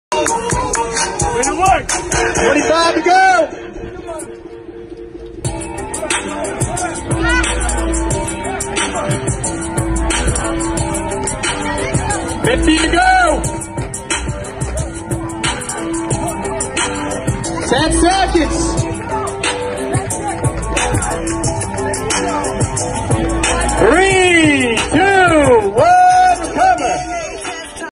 Inertia Wave
The Inertia Wave is far superior to the battle ropes blowing them away in core engagement, energy systems, caloric expenditure, and cognitive skills.